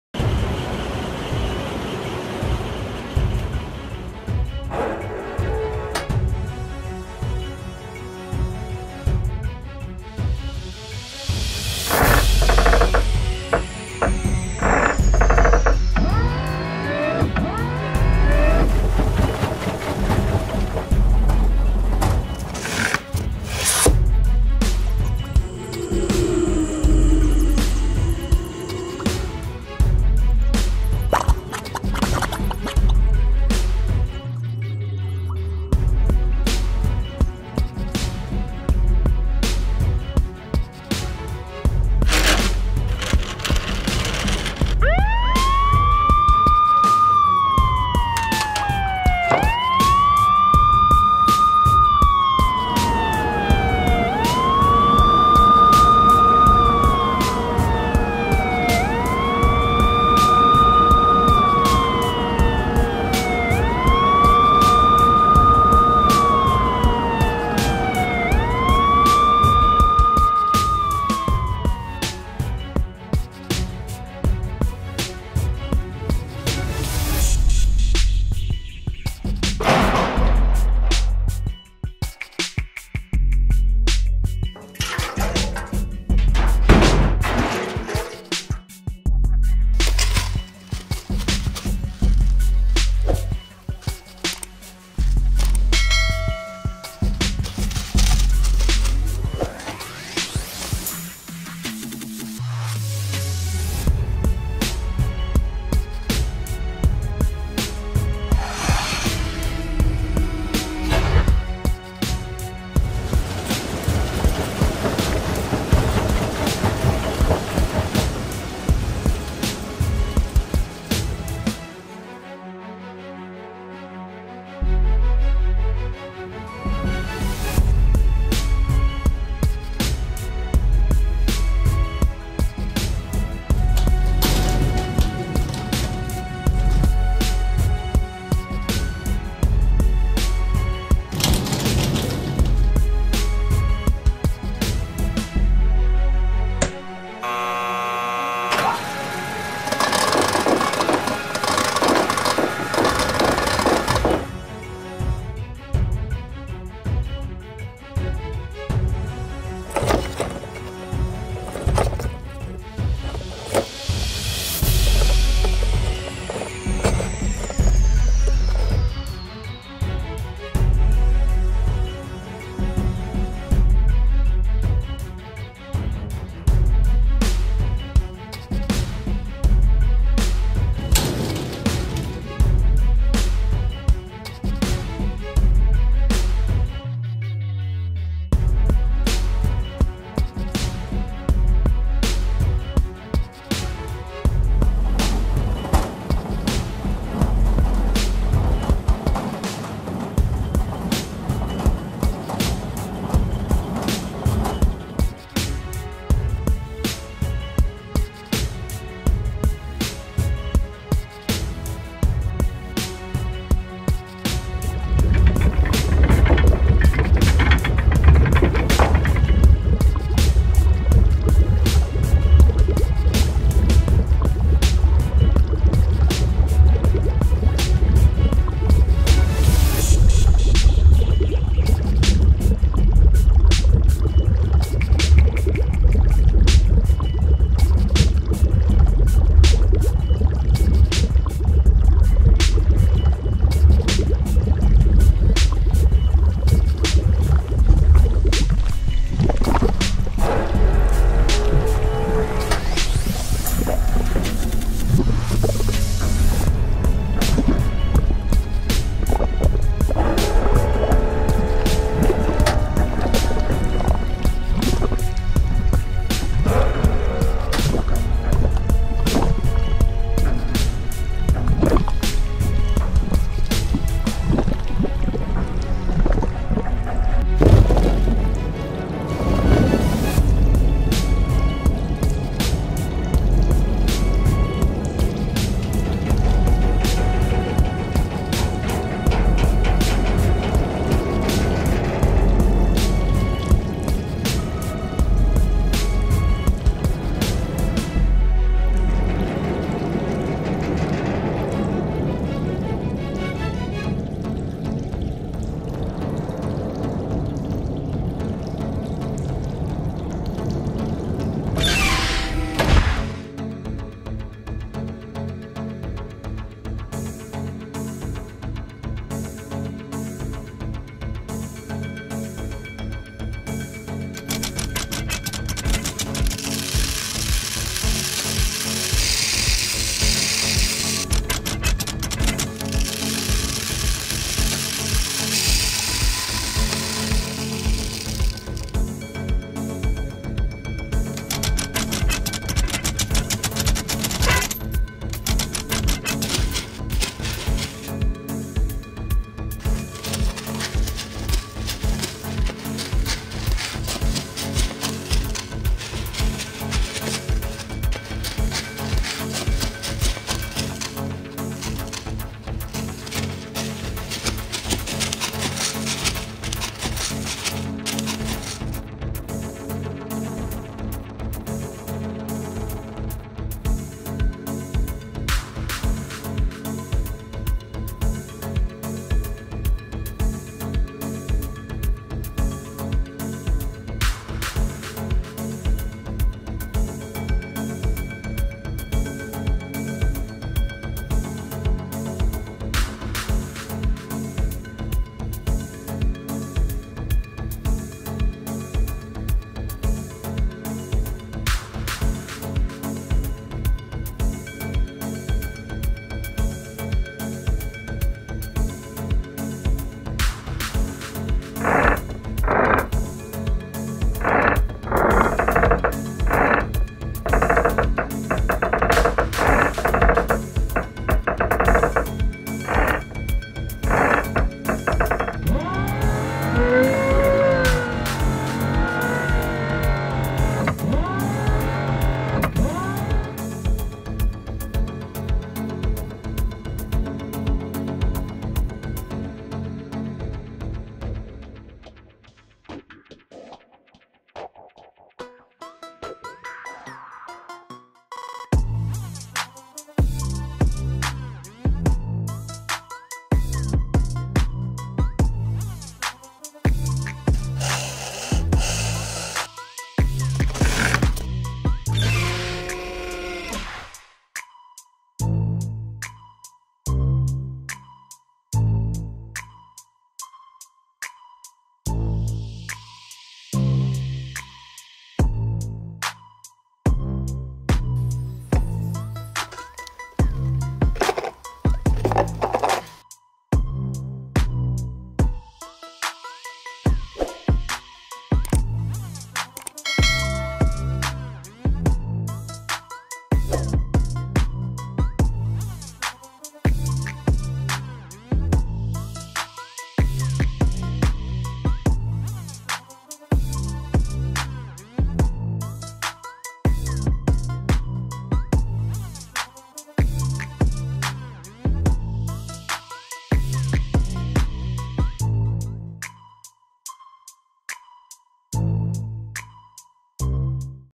🐹 Hamster Escapes the Creative Maze for Pets in real life 🐹 in Hamster Stories #1
New maze for cute hamster in real life! It’s a part of Hamster Stories where my funny pet hamster was sent to the prison for pets. Is hamster Popcorn brave and fast enough to get out of dangerous traps end escape from pets police?